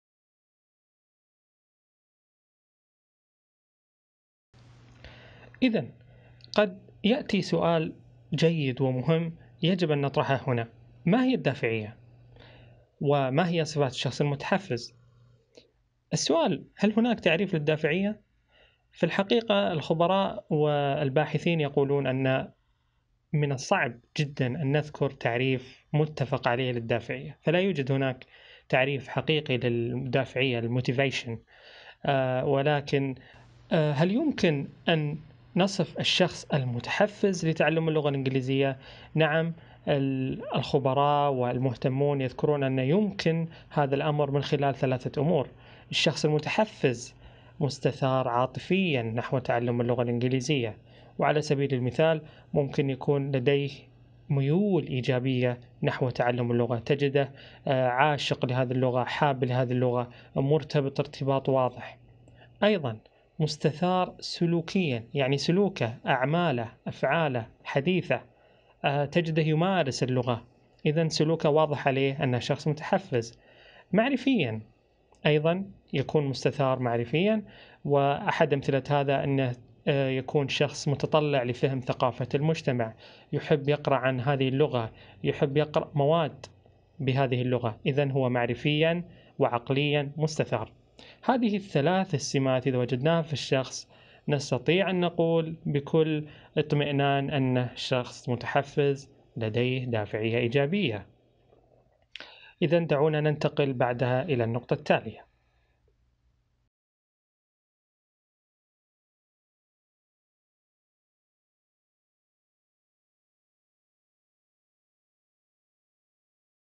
رواق: كيف تحفز نفسك بفاعلية نحو تعلم اللغة الإنجليزية - 2 - 2
رواق: كيف تحفز نفسك بفاعلية نحو تعلم اللغة الإنجليزية، المحاضرة الثانية، الجزء الثاني: ماهي الدافعية وماهي صفات الشخص المتحفز